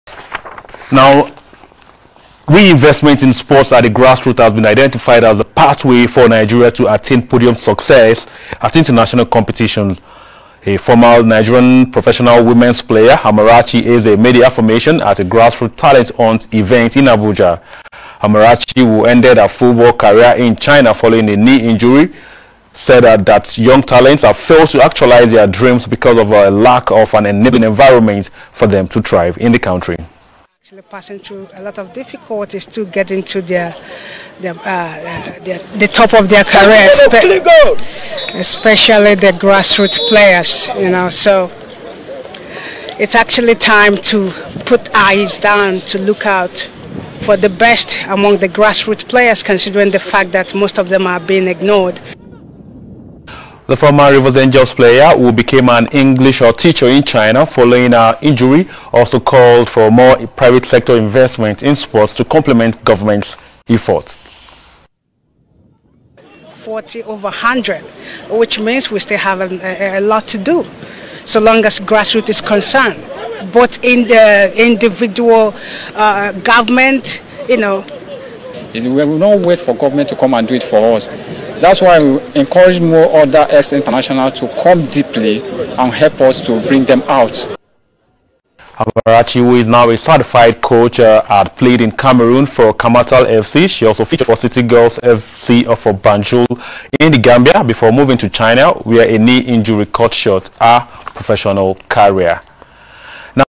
LILY LEGENDS FOOTBALL ACADEMY AIT SPORTS NEWS INTERVIEW WITH THE LEGENDS CEO AMARACHI LILIAN EZE
Following my efforts in making sure that the grassroots players are giving opportunities to showcase their Talents, I granted an interview with the AIT SPORTS NEWS where i encouraged individual and governments to invest in grassroots players. Teams around the country to give the Grassroots players opportunities to showcase their talents without sentiments.